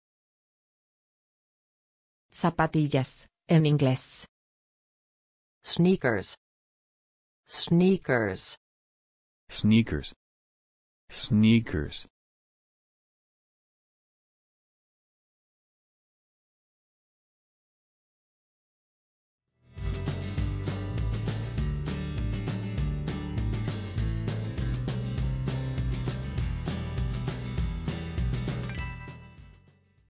Cómo se dice zapatillas en inglés
¿Cómo se dice correctamente zapatillas en inglés? Escuche con nosotros.
¿Cuál es la traducción correcta de la palabra zapatillas del español al inglés? Habla como los americanos. Además, ¿cómo escribir correctamente la palabra sneakers?

¡Gracias!